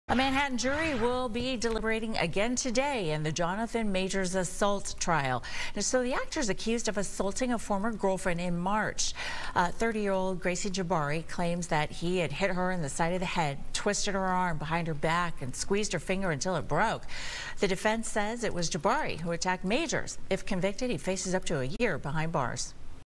Jonathan Majors assault trial: Deliberations continue
A Manhattan jury will be deliberating again today in the Jonathan Majors assault trial.